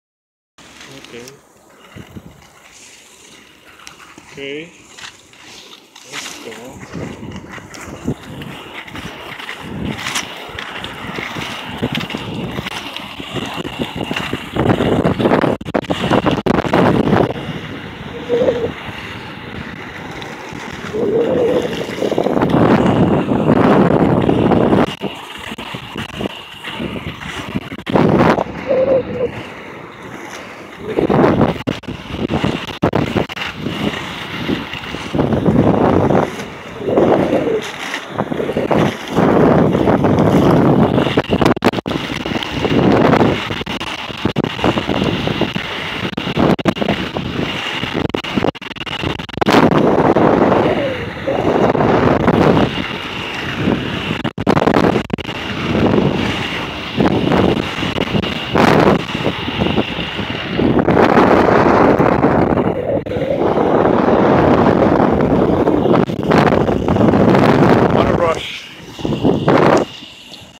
Testing the MPC Wheels
Skating at fort tyron park
1 mile